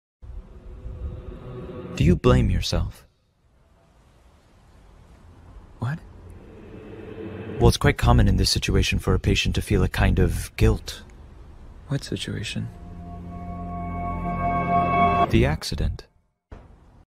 the accident // animation meme
I liked this audio a lot and saw a lot of people animating it and wanted to do one too. I've never animated this one oc that I bought from my friend a long time ago (Doge) but I wanna doodle and animate him more, starting with this one uwu